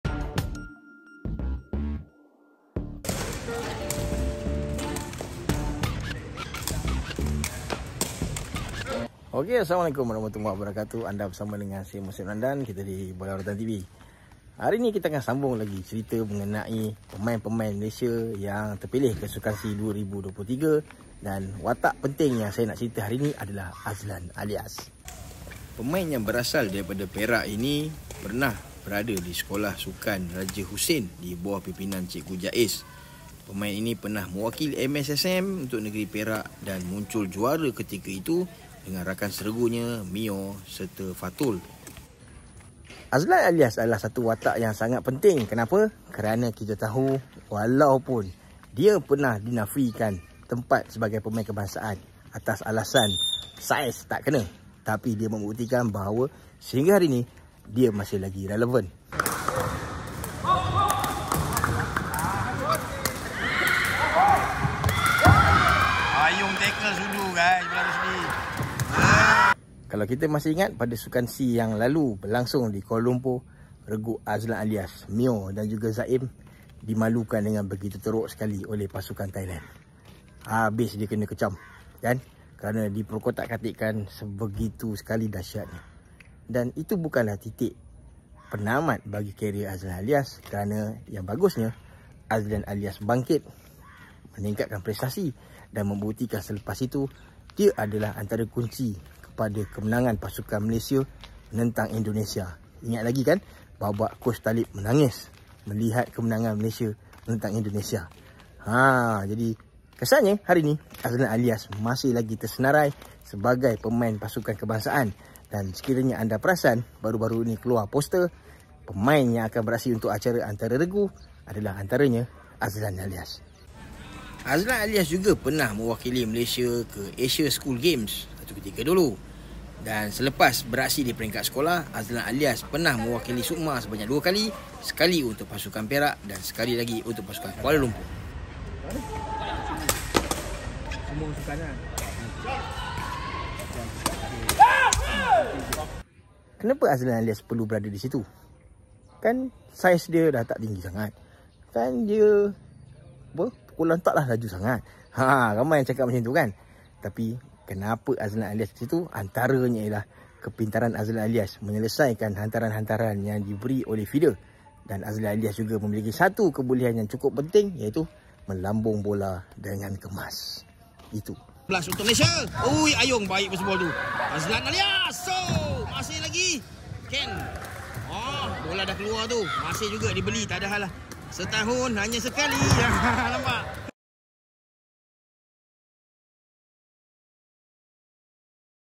Kenapa Azlan Alias?
Walaupun ramai yang mempertikai Azlan Alias terpilih ke Sukan SEA 2023, kita perlu faham dan terima bahawa Azlan Alias masih antara Killer terbaik negara ketika ini.
Semoga sukses Azlan Alias!